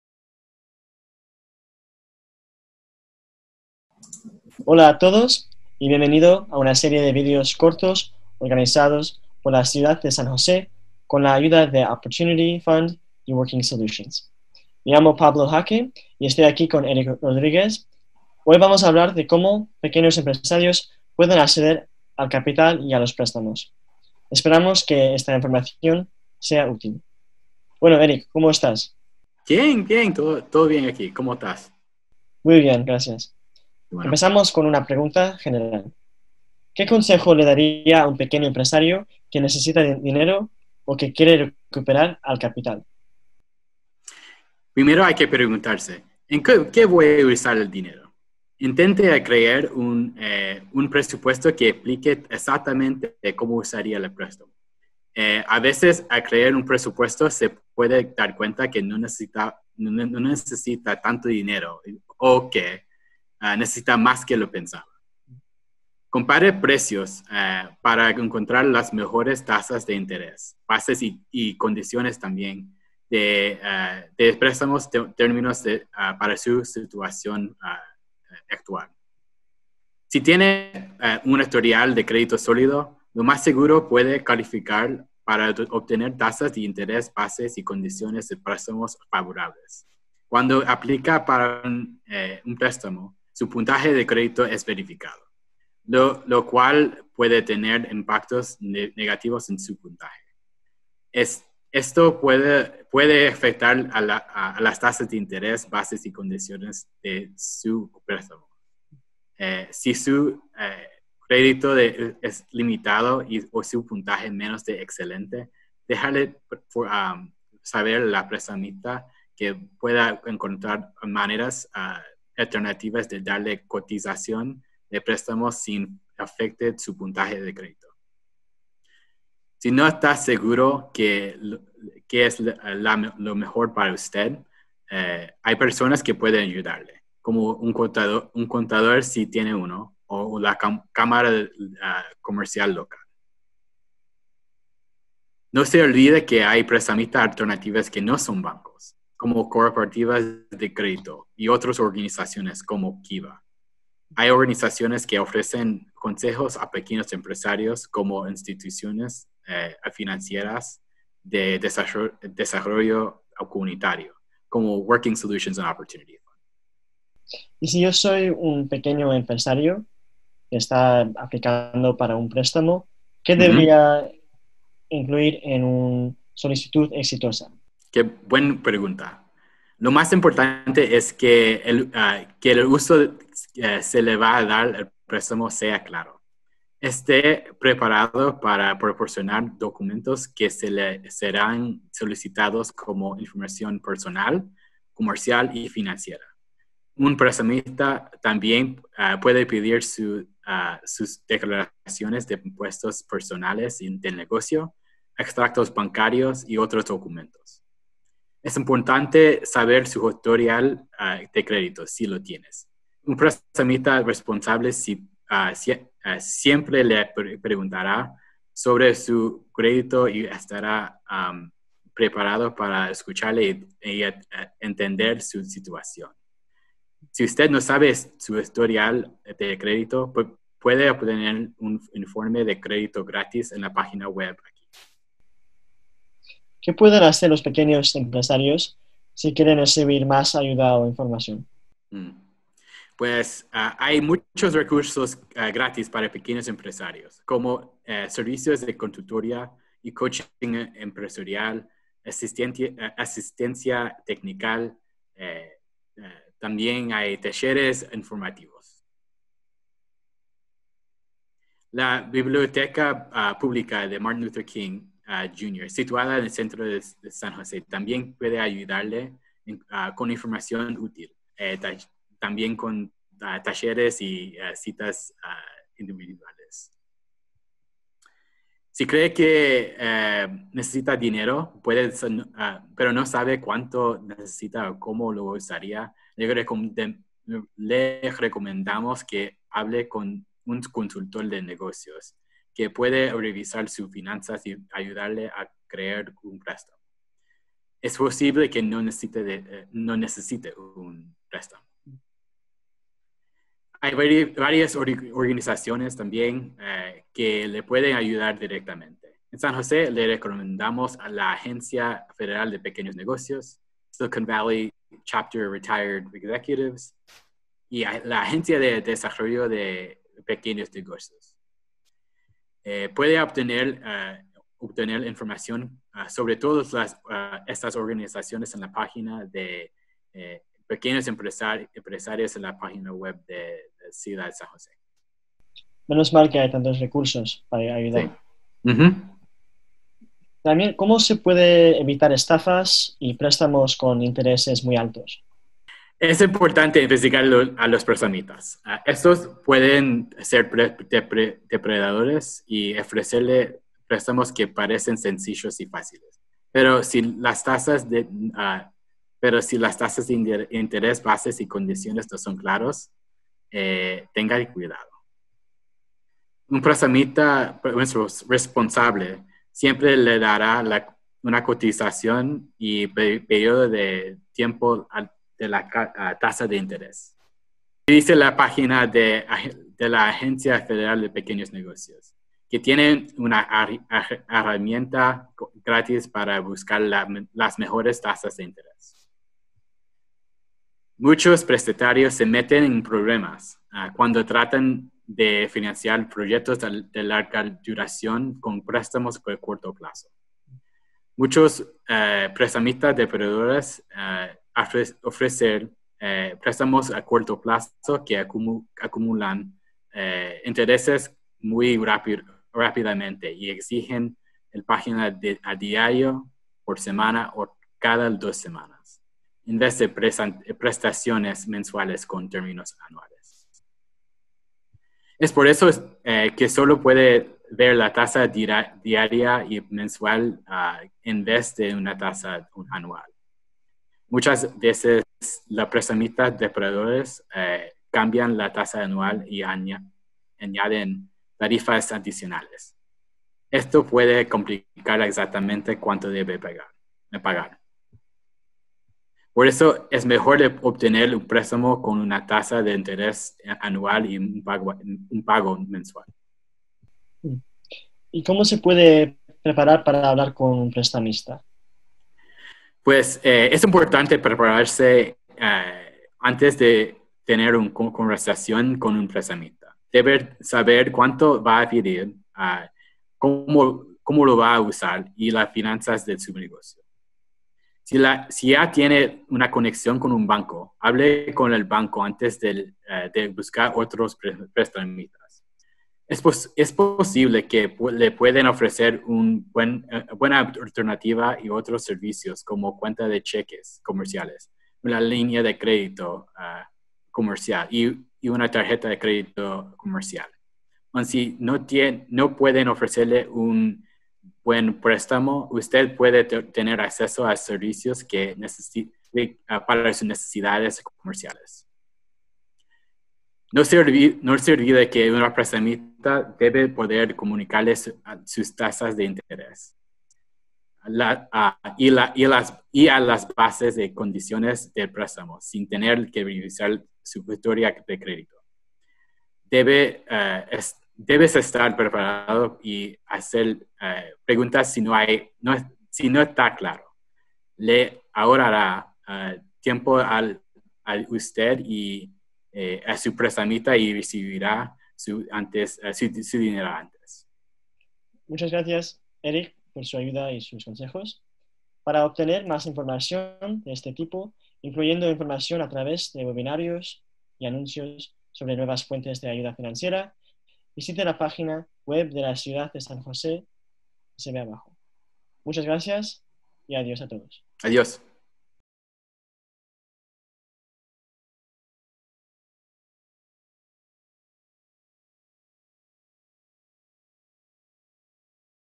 Apoyo financiero para pequeñas empresas
Pablo Haake y Eric Rodriguez de la Ciudad de San José presentan información sobre cómo los pequeños empresarios pueden encontrar y solicitar apoyo financiero para enfrentar el impacto de COVID-19
00:37 ¿Qué consejo general le daría a un pequeño empresario que necesita dinero o que quiere recuperar al capital?
2:48 ¿Qué debería incluir una solicitud exitosa?
3:56 Que pueden hacer los pequeños empresarios si quieren recibir mas ayuda?
5:36 ¿Cómo se puede evitar estafas y préstamos con intereses altos?
7:38  ¿Cómo se puede preparar para hablar con un prestamista?

Para más información por favor visite:
La Oficina de Desarrollo Económico de la Ciudad de San José